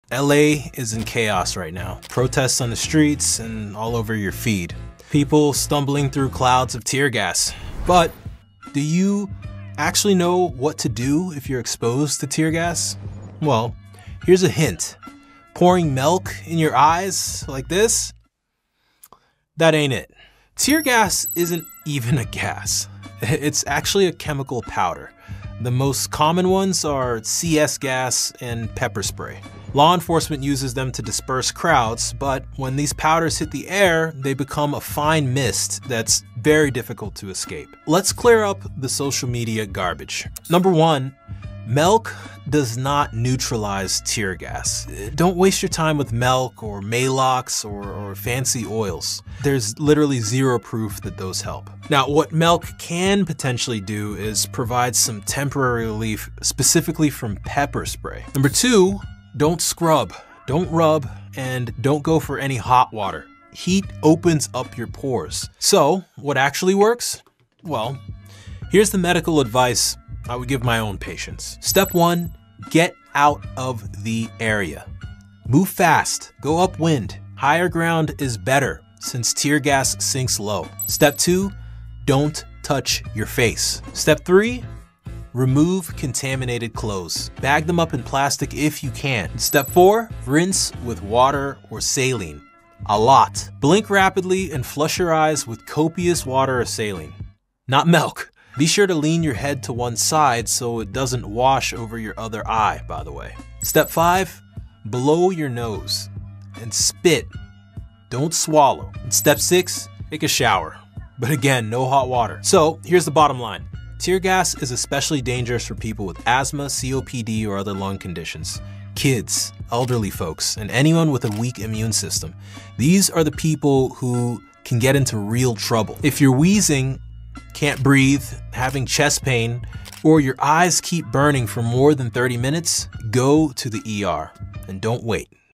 STOP using milk for TEAR GAS. Doctor Explains the real treatment
Tear gas is everywhere in LA right now. You’ve seen people pour milk in their eyes—does it actually work? Here’s what really happens after exposure, and what doctors actually recommend.

Watch till the end for the facts!